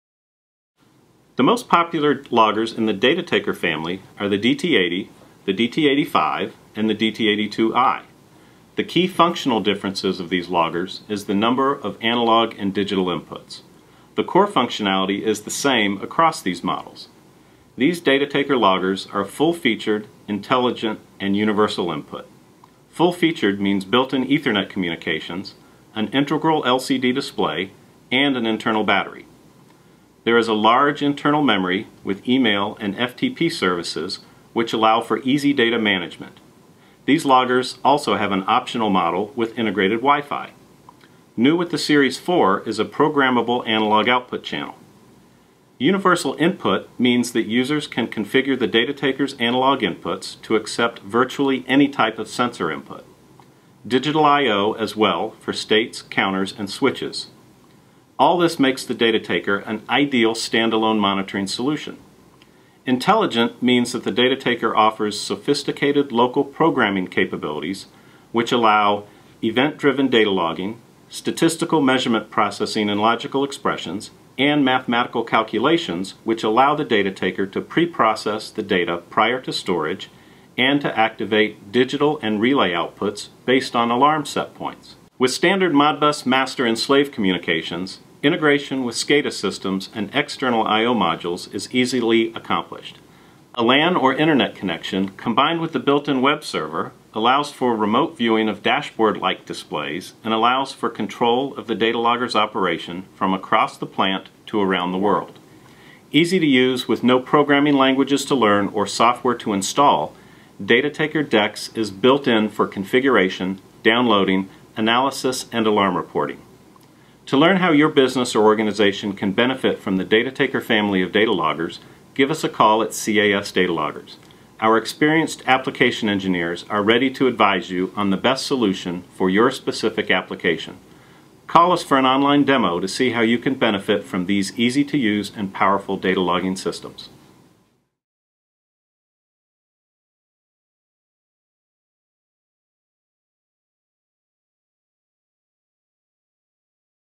dataTaker Intelligent, Universal Input Family of Loggers
The most popular loggers in the dataTaker family are the DT80, the DT85 and the DT82i. The key functional differences of these loggers is the number of analog and digital inputs. The core functionality is the same across these models.

These dataTaker loggers are full-featured intelligent, universal input. Full-featured means built-in Ethernet communications, an integral LCD display, an internal battery. There is a large, internal memory with email and FTP services which allows for easy data management. These loggers have optional models with integrated WiFi. New with the Series 4 is a programmable analog output channel.